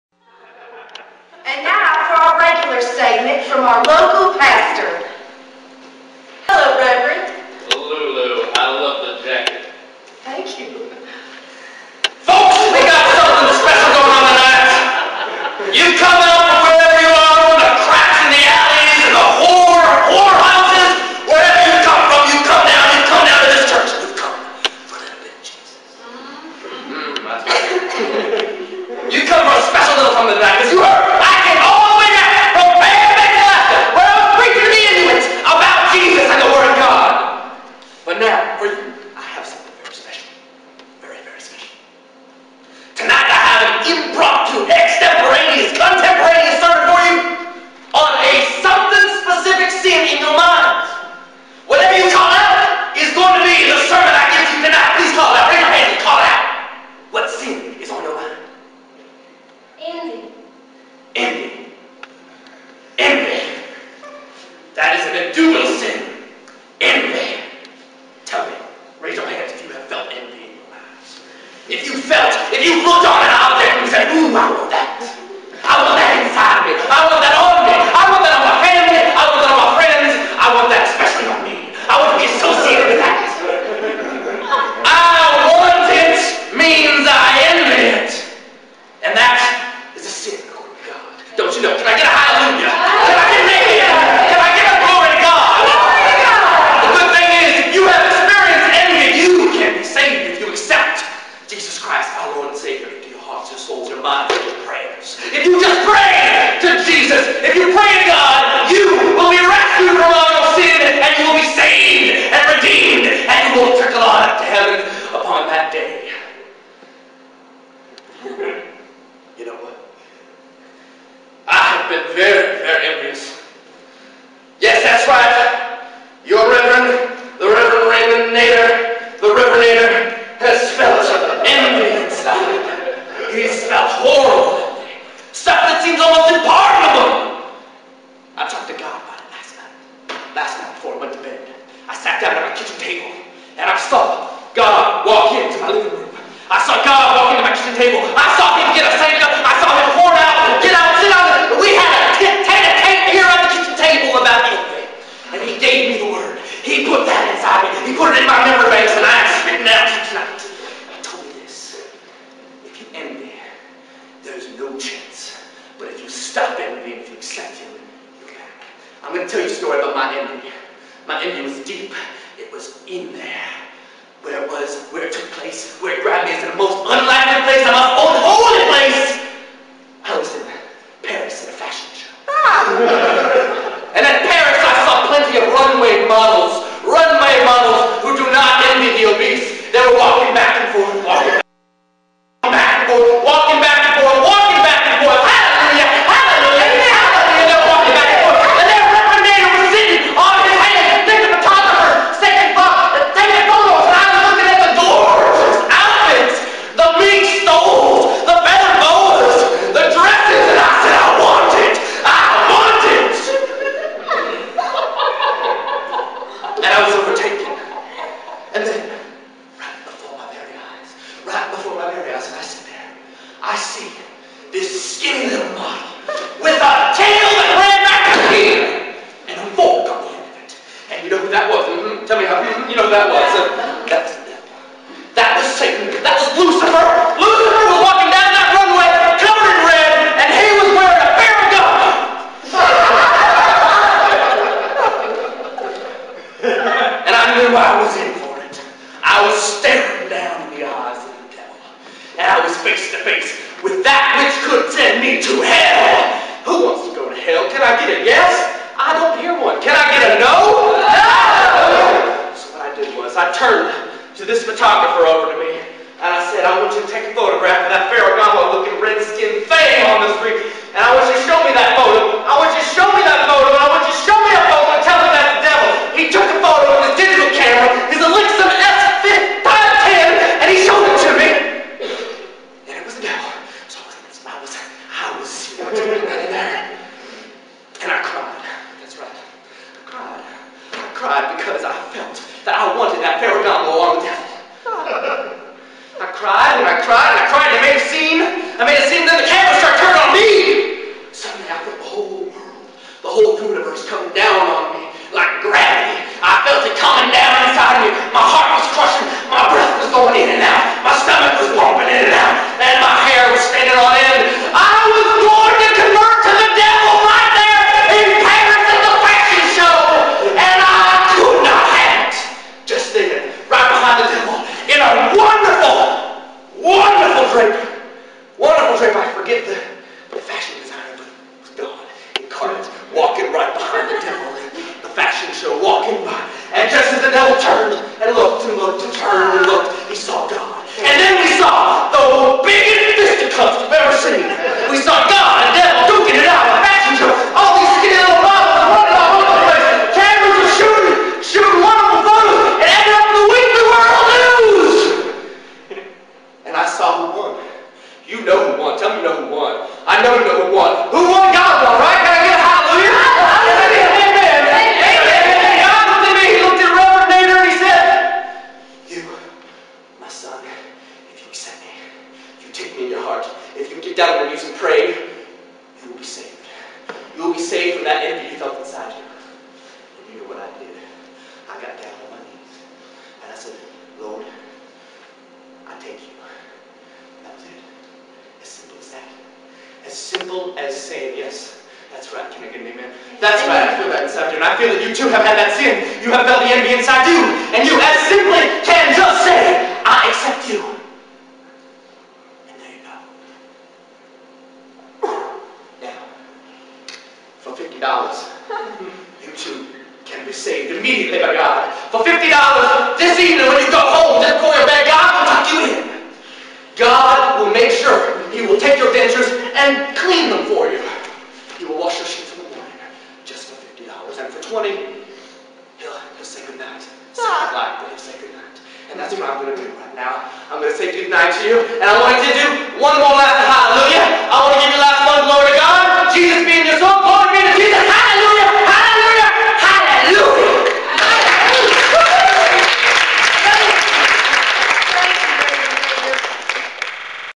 "Envy" - An Improvised Sermon by The Reverend Raymond Nader
The debut performance of The Reverend Raymond Nader (aka Ben Hauck) doing a completely improvised, audience-inspired sermon in the musical sketch comedy show Ding Dang!, based on the suggestion "envy."

That's Casey Weaver introducing The Reverend in the beginning.

Date: 11/18/2006